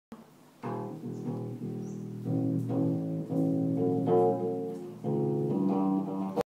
Bass Guitar
For sale in a good condition.